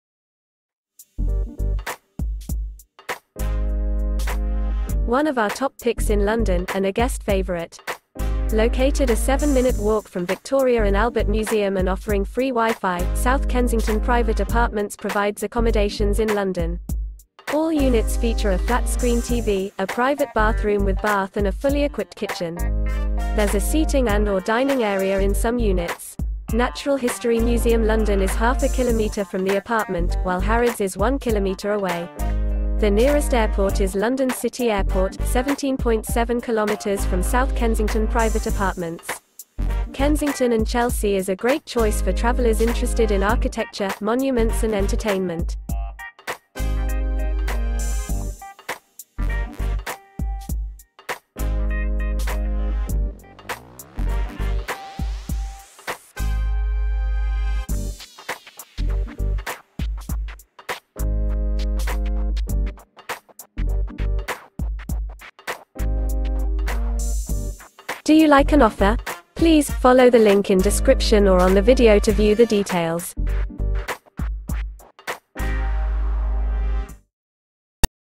South Kensington Private Apartments | Review Hotel in London, Great Britain
One of our top picks in London – and a guest favorite. Located a 7-minute walk from Victoria and Albert Museum and offering free WiFi, South Kensington Private Apartments provides accommodations in London. All units feature a flat-screen TV, a private bathroom with bath and a fully equipped kitchen. There's a seating and/or dining area in some units. Natural History Museum London is half a kilometer from the apartment, while Harrods is 1 km away. The nearest airport is London City Airport, 17.7 km from South Kensington Private Apartments. Kensington and Chelsea is a great choice for travelers interested in architecture, monuments and entertainment.